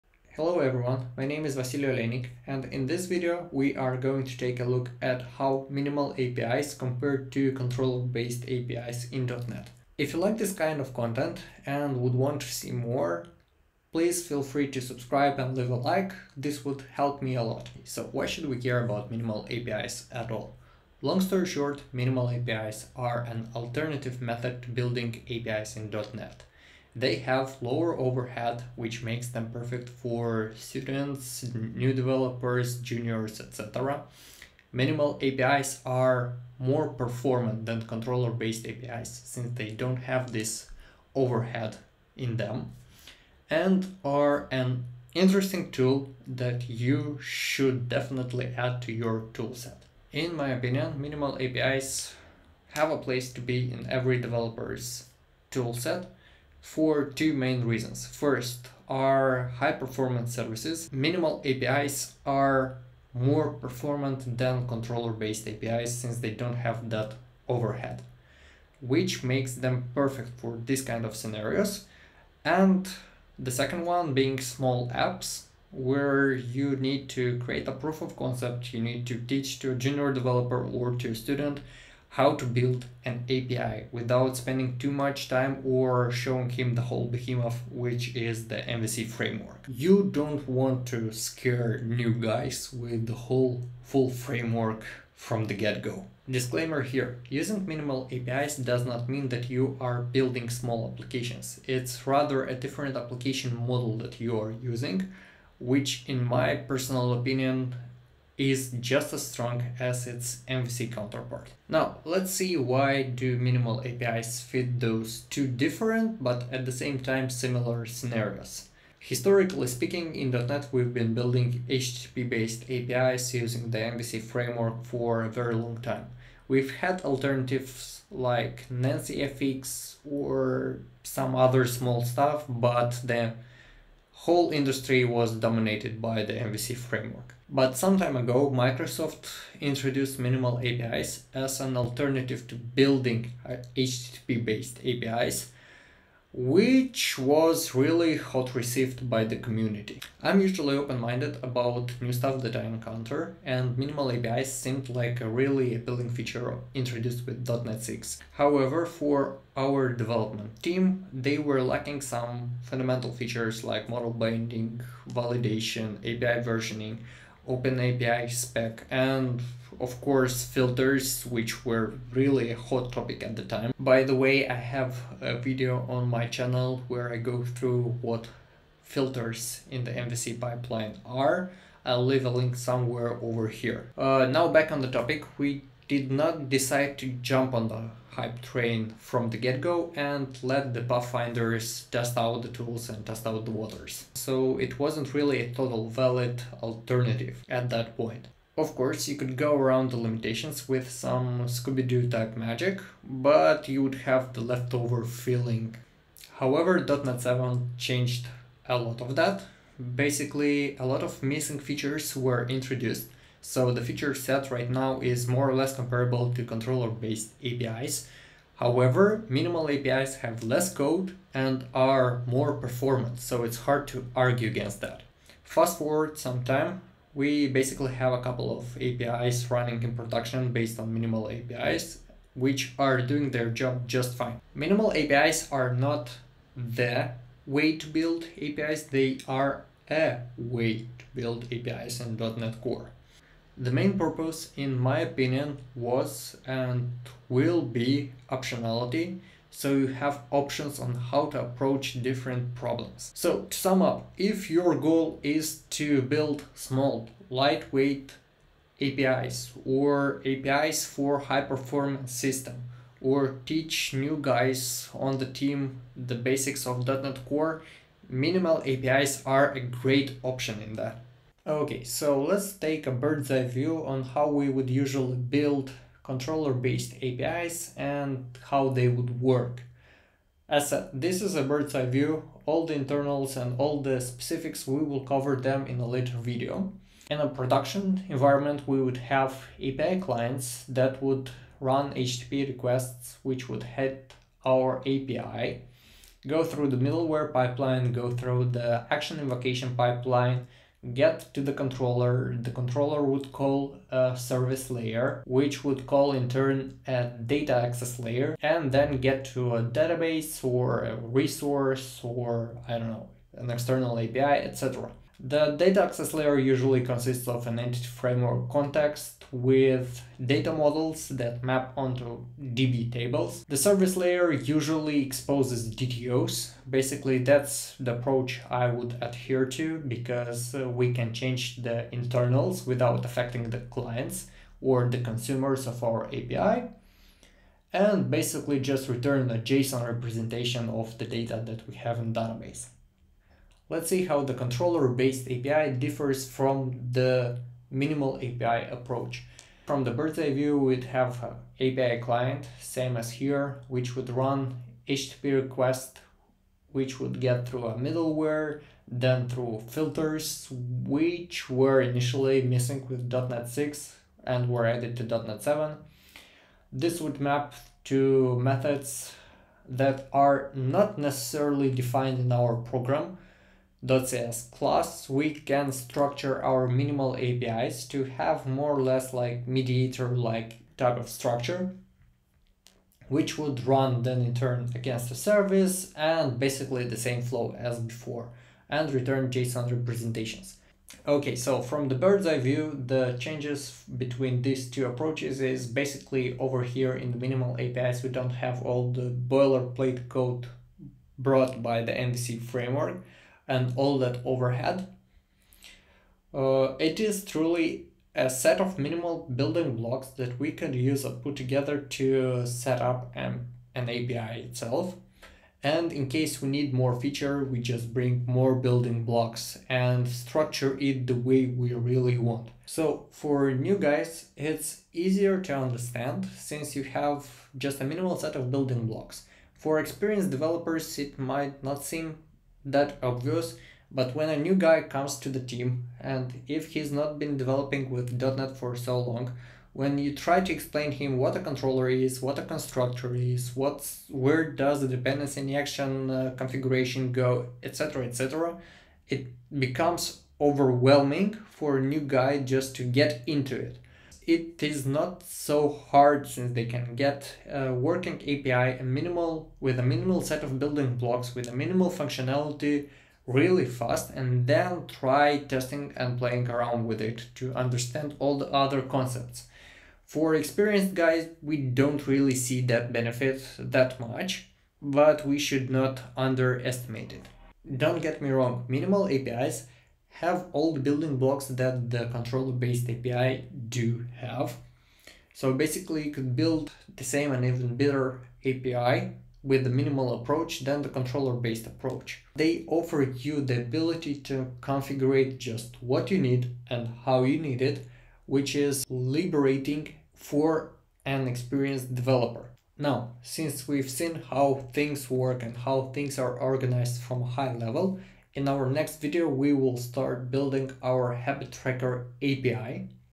.NET Minimal API Part 2 - Comparison to Controller based API's
This is the second video in the Habit Tracker Minimal API series.
In this video we will go over how Minimal API's compare to the 'traditional' Controller based API's and why were Minimal API's introduced in the first place.

0:00 Why should you care about Minimal API's
0:50 Main use cases
3:56 A new way to building API's
4:33 Controller based API vs Minimal API from birds eye view
6:40 Comparison Conclusion